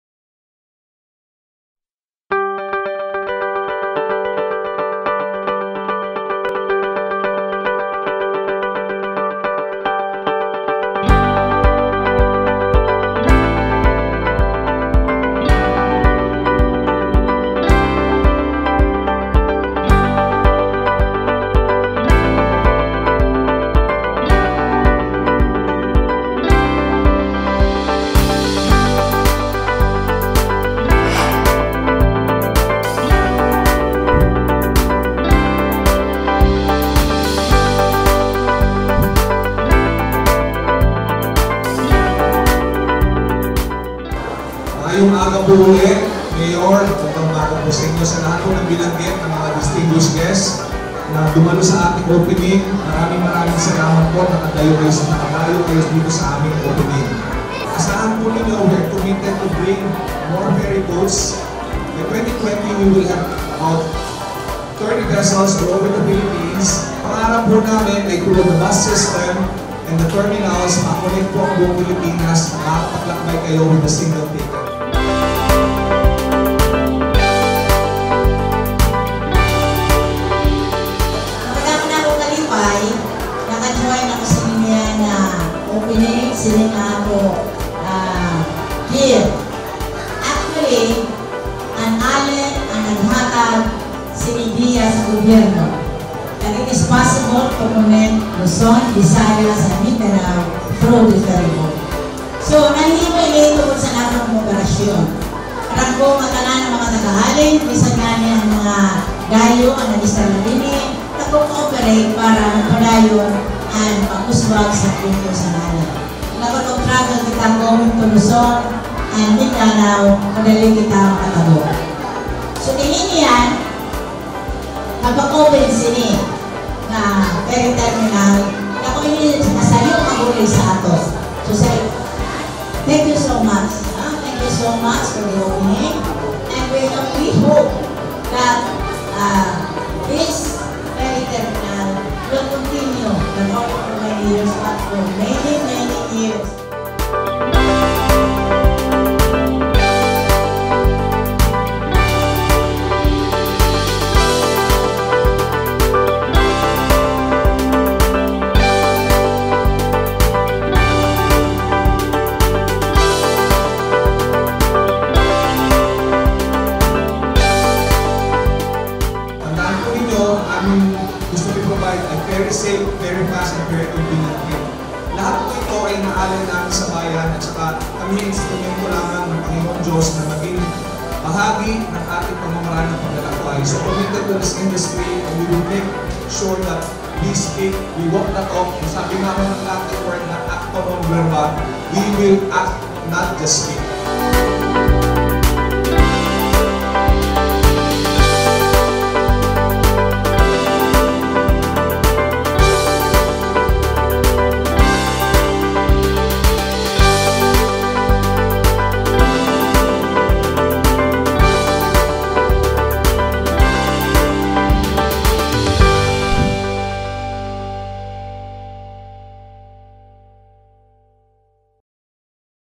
Matnog Dapdap Launching
A FerryGreat day to all! Here's the ceremonial blessing of Matnog-Dapdap route. To know more about the routes of FastCat nationwide please